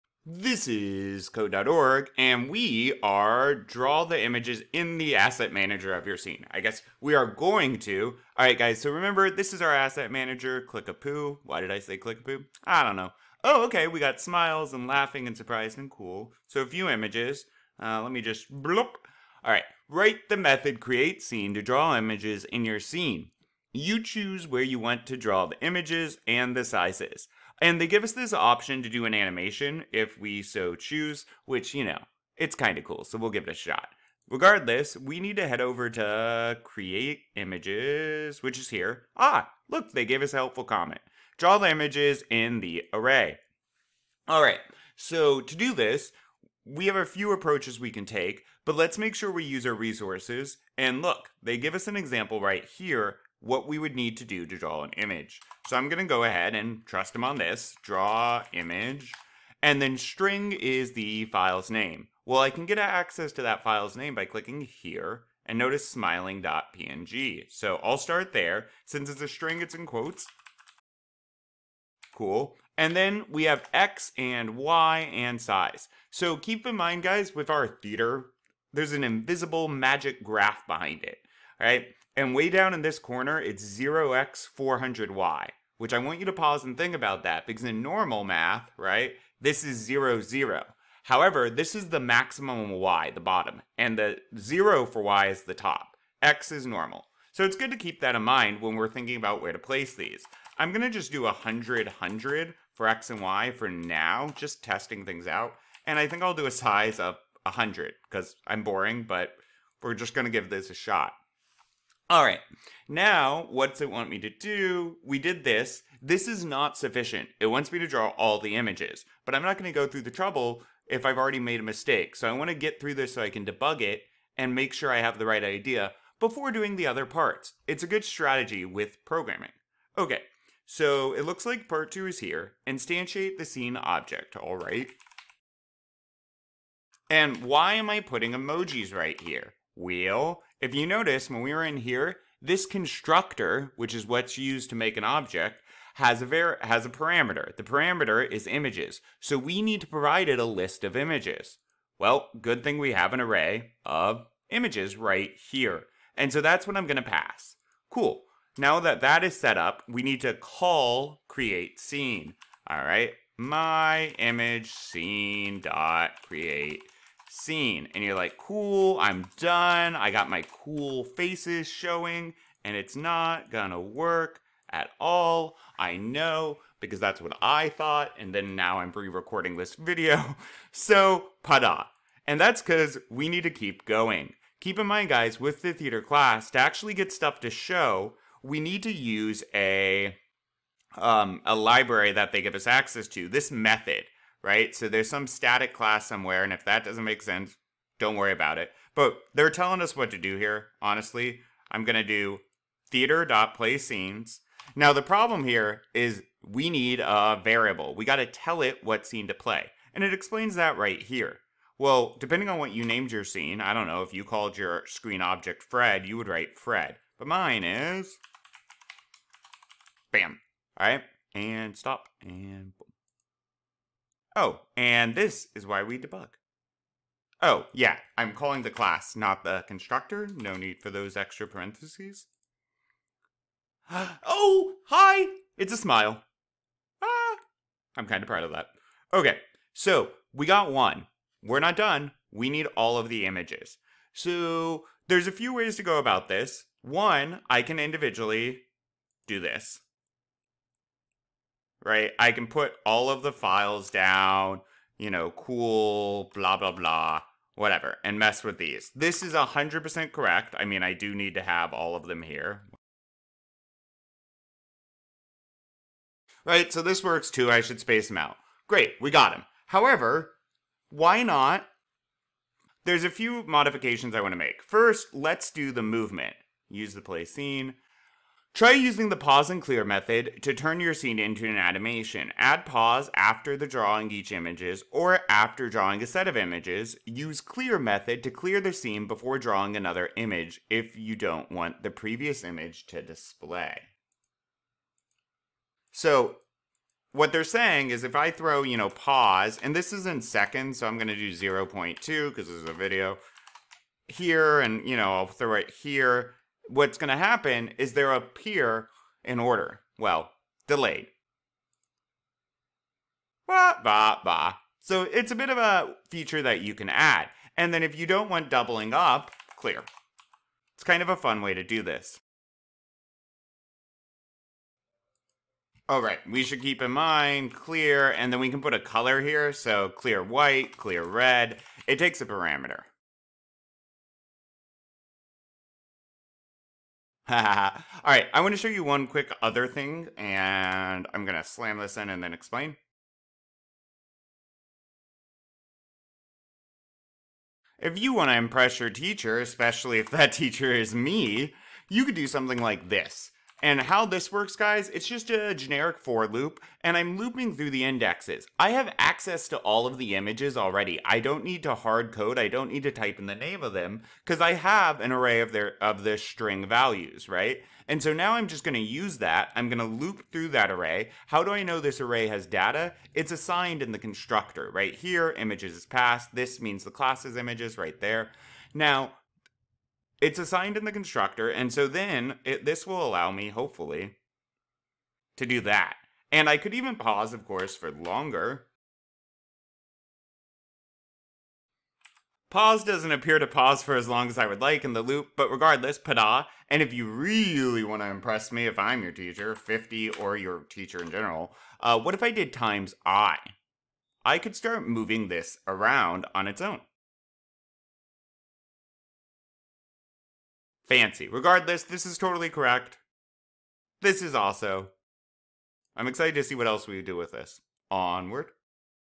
Code.org Lesson 1.3 The Theater | Tutorial with Answers | Unit 4 Computer Science A
Conditions and Logic is unit 4 of Code.org's Computer Science A (CSA) course. The course is often used in AP Computer Science classrooms. Students utilize Java (programming language) to accomplish tasks by designing, writing, and testing computer programs. In this video, I walk through the step-by-step how to get the right answer for this puzzle.